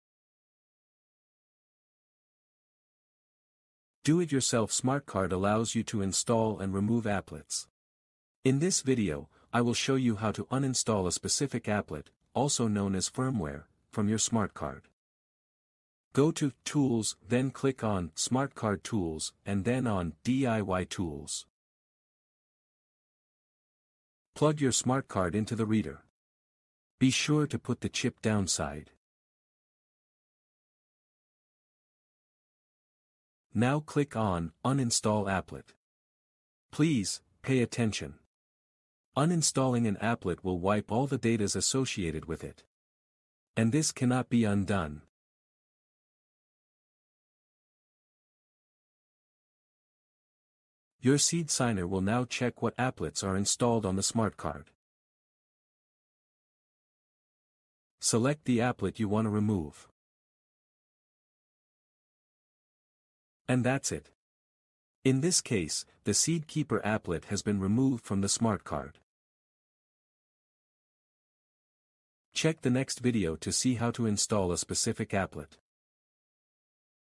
Satochip x SeedSigner - How to remove an applet from the DIY smartcard
🧹 Need to start fresh with your smartcard? In this video, I’ll show you how to safely remove an applet from your DIY smartcard so you can reload, reset, or upgrade it with ease.

👉 Whether you’re experimenting with different applets or just cleaning up, this guide has you covered.